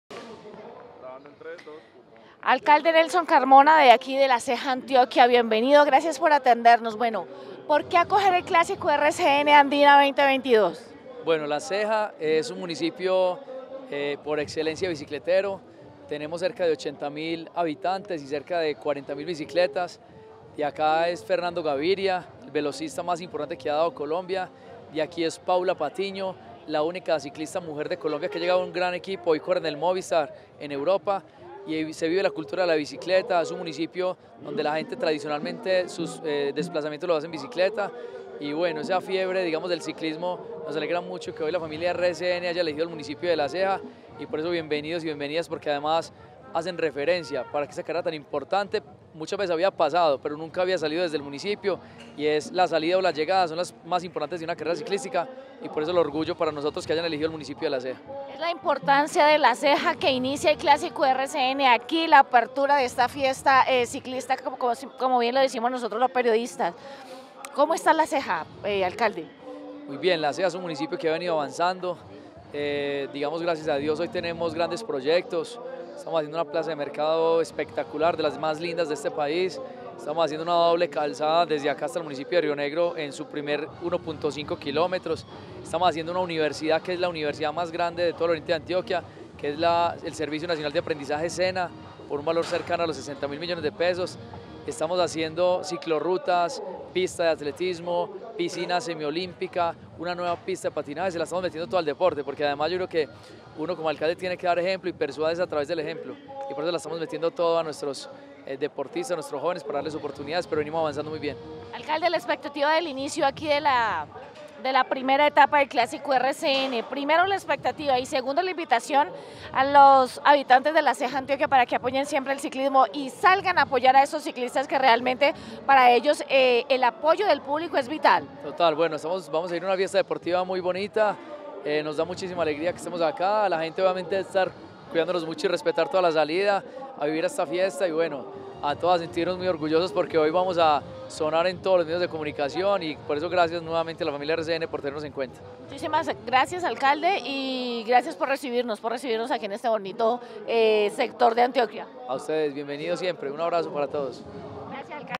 JOHAN COLON DEL ORGULLO PAISA HACE DOBLETE EN LA APERTURA DEL CLÁSICO RCN(3)
JOHAN COLON DEL ORGULLO PAISA HACE DOBLETE EN LA APERTURA DEL CLÁSCIO RCN

ANÍBAL GAVIRIA GOBERNADOR DE ANTIOQUIA DIO LA PARTIDO A LA PRIMERA ETAPA DEL “DUELO DE TITANES”

Una etapa exigente que concluyó en Rionegro Antioquia y en la que se impuso Johan Colón del Orgullo Paisa, dio apertura hoy al Clásico RCN Cerveza Andina 2022.

En la salida de la etapa estuvo presente y dio el banderazo inicial el gobernador de Antioquia Aníbal Gaviria, quien estuvo acompañado por Fernando Molina presidente de RCN radio y Héctor Palau Saldarriaga director del Clásico RCN, además de personalidades importantes de la región

La fracción de 154 Km transcurrió sobre un terreno muy quebrado en el que no faltaron las fugas en las que se destacaron Walter Pedraza del Nectar Cundinamarca y Robigzon Oyola del Team Medellín, a la postre líderes de la montaña y los sprint especiales.

En el remate de la etapa el lote se juntó y provocó un embalaje masivo en que con autoridad se impuso Johan Colón, quien dé pasó se convirtió en el primer líder del “duelo de titanes”.

Diego Ochoa del EPM Scott y Brayan Sánchez del Team Medellín, secundaron a Colón en la línea de meta y también le siguen en la clasificación general individual.

La carrera continuará mañana domingo con una etapa de 150,8 Km entre Medellín con final en Maceo, premio de montaña de segunda categoría, donde se podrían ver las primeras diferencias serias en la clasificación general.

Clasificaciones

Clasificación etapa 1

1.Johan Colon                  ORGULLO PAISA ANT        04:01:57

2.Diego Ochoa                  EPM-SCOTT                                  mt.

3.Brayan Sánchez            TEAM MEDELLÍN EPM                     mt.

4.Adrian Bustamante        SISCREDITO-GW                          mt.

5.Jeison Casallas              COL TIERRA DE ATLETAS              mt.

6.Juan Tito Rendon           ORGULLO PAISA                  mt.

7.Yonder Godoy                JB. A ZULIA-ARDUVI                        mt.

8.Alexander Gil                  ORGULLO PAISA                             mt.

9.Walter Pedraza                NECTAR IND-WCARGO                  mt.

10.Rodrigo Contreras       EPM-SCOTT                  mt.

Clasificación general

1.Johan Colon                  ORGULLO PAISA ANT        04:01:47

2.Diego Ochoa                  EPM-SCOTT               a 4

3.Brayan Sánchez            TEAM MEDELLÍN EPM                    a 6

4.Robigzon Oyola      TEAM MEDELLÍN EPM                    a 7

5.Aldemar Reyes              TEAM MEDELLÍN EPM                    a 7

6.Hernan Aguirre      SISCREDITO-GW                            a 8

7.Wilson Peña           COL TIERRA DE ATLETAS             a 8

8.Walter Pedraza               NECTAR IND-WCARGO                 a 9

9.Wilson Peña           COL TIERRA DE ATLETAS             a 9

10.Adrian Bustamante       SISCREDITO-GW                         a10